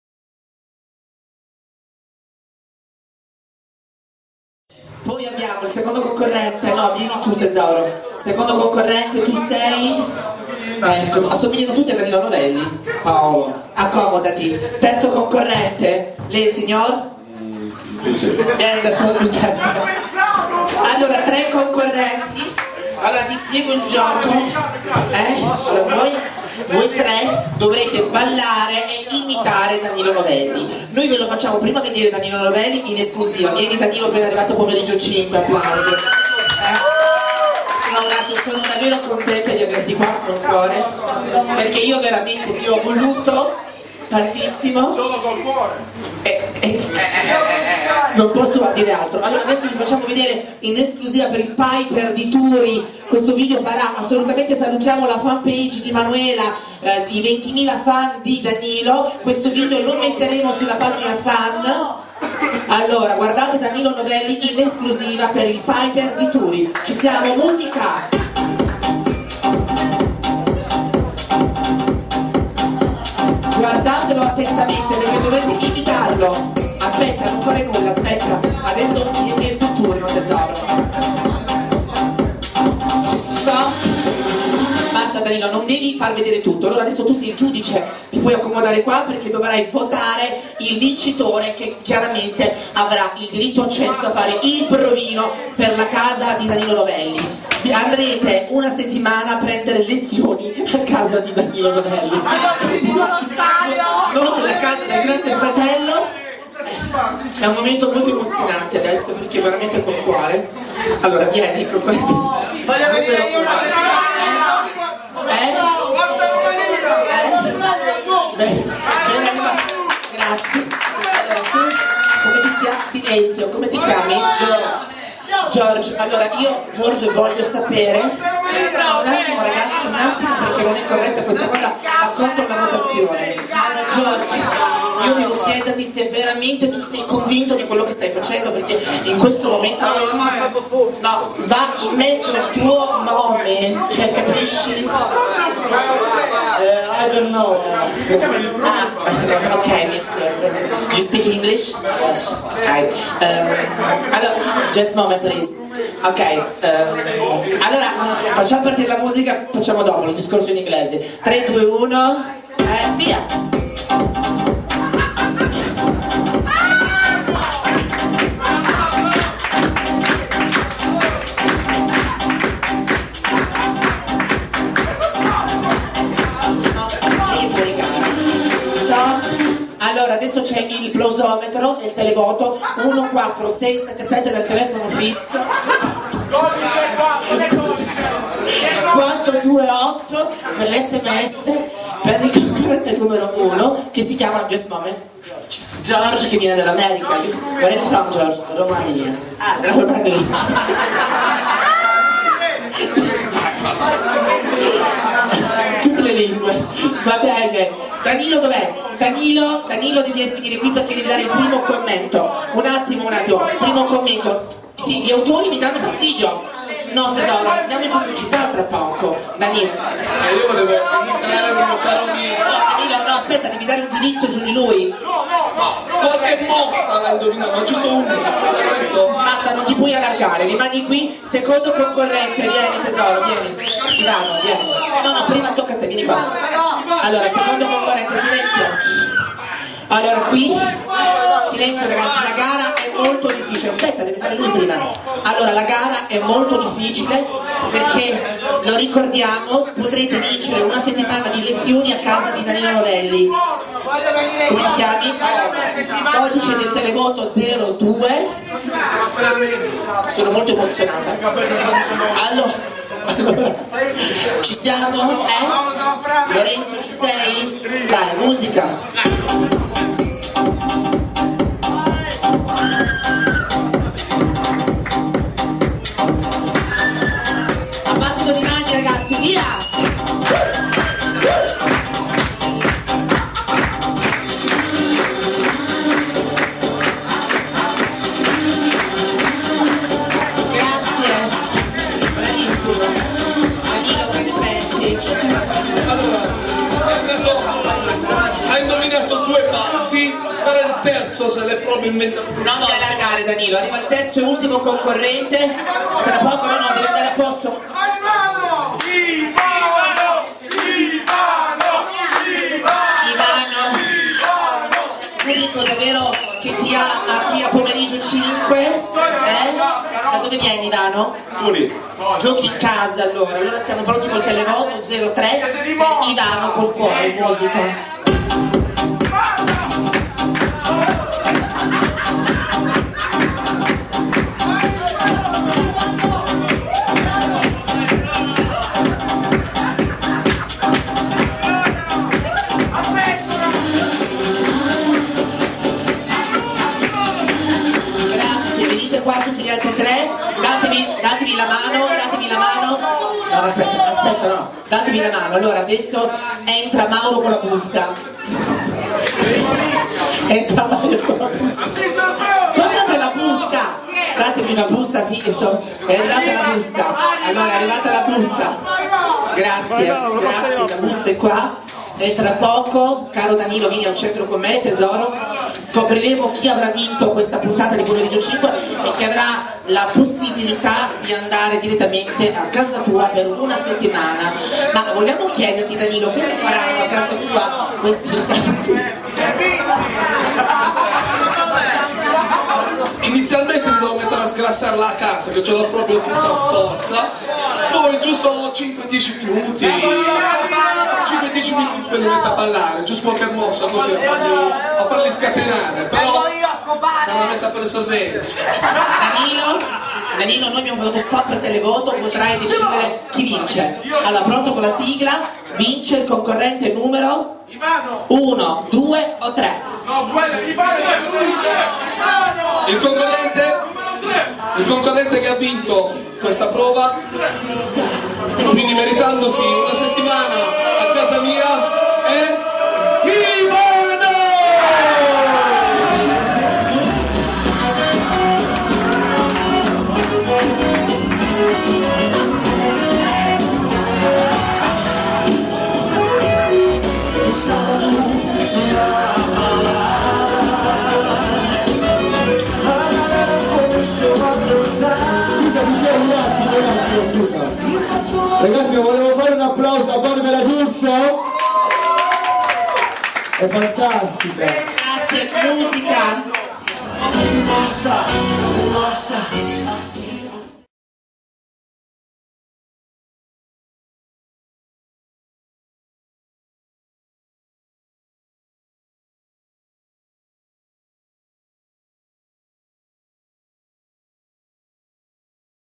WeekEnd Vip 17 PIPER Turi M.G. Management : imitazione di Danilo Novelli
WeekEnd Vip 17 PIPER Turi M.G. Management : Gabrio Gamma, Ludovica Chieffo,Danilo novelli,Ylenia Isidori a Bluradio , Controradio Bari , studi nel Bari Blu Triggiano ,sportphotopress video